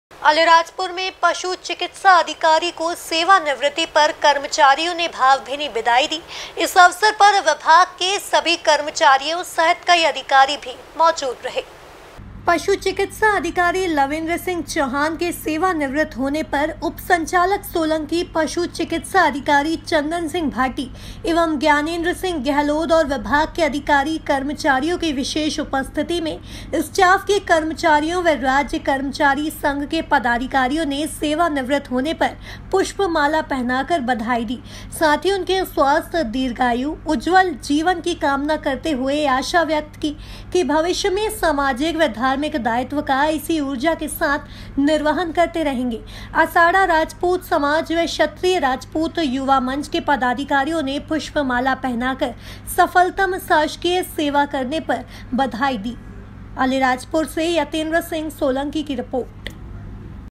अलीराजपुर | पशु चिकित्सा अधिकारी हुए सेवा निवृत | वर्षो के सेवाकाल से अभिभूत सभी ने किया स्वागत
पशु चिकित्सा अधिकारी लविंद्रसिंह चौहान के सेवानिवृत्त होने पर उपसंचालक सोलंकी पशु चिकित्सा अधिकारी चंदन सिंह भाटी एवं ज्ञानेंद्रसिंह गेहलोद ओर विभाग के अधिकारी कर्मचारियों की विशेष उपस्थिति में स्टॉफ के कर्मचारियों व राज्य कर्मचारी संघ के पदाधिकारियों ने सेवानिवृत्त होने पर पुष्प माला पहनाकर बधाई दी साथ ही उनके स्वस्थ,  दीर्घायु , उज्जवल जीवन की कामना करते हुए यह आशा व्यक्त की भविष्य मे सामाजिक व धार्मिक दायित्व का इसी ऊर्जा के साथ निर्वहन करते रहेंगे। असाडा राजपूत समाज व क्षत्रिय राजपूत युवा मंच के पदाधिकारियों ने पुष्प माला पहनाकर सफलतम शासकीय सेवा करने पर बधाई दी। अलीराजपुर से यतेंद्र सिंह सोलंकी की रिपोर्ट

फेसबुक

इंस्टाग्राम

ट्विटर